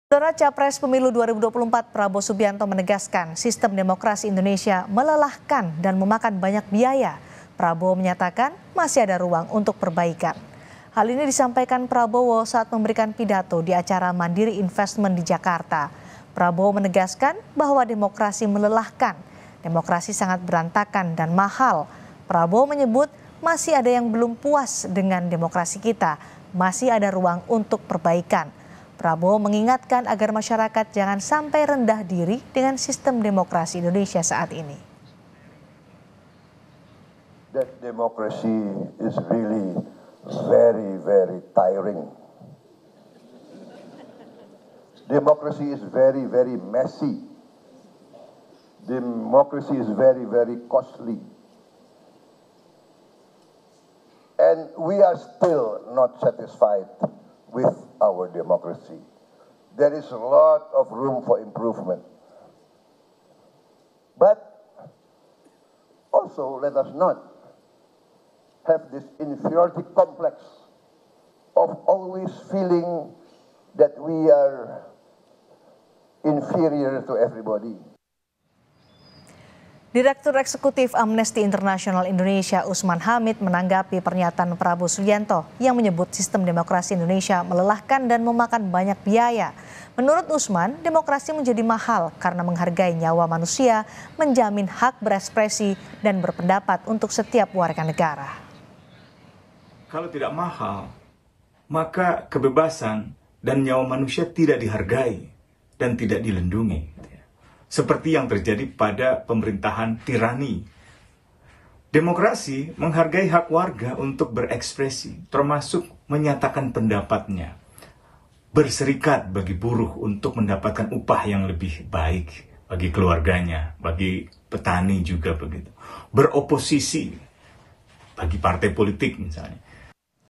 Tanggapi Pernyataan Prabowo Soal Demokrasi, Usman Hamid: Demokrasi Mahal Karena Jamin Hak Manusia
KOMPAS.TV - Calon Presiden Pemilu 2024, Prabowo Subianto menegaskan sistem demokrasi Indonesia melelahkan dan memakan banyak biaya. Prabowo menyatakan masih ada ruang untuk perbaikan.

Direktur Eksekutif Amnesty International Indonesia, Usman Hamid menanggapi pernyataan Prabowo Subianto tersebut.

Prabowo menegaskan bahwa demokrasi melelahkan. Demokrasi sangat berantakan dan mahal. Prabowo menyebut masih ada yang belum puas dengan demokrasi kita. Masih ada ruang untuk perbaikan. Prabowo mengingatkan agar masyarakat jangan sampai rendah diri dengan sistem demokrasi saat ini.

Direktur Eksekutif Amnesty International Indonesia, Usman Hamid menanggapi pernyataan Prabowo Subianto, yang menyebut sistem demokrasi indonesia melelahkan dan memakan banyak biaya. Menurutnya demokrasi jadi mahal karena menghargai nyawa manusia, menjamin hak berekspresi, dan berpendapat setiap warga negara.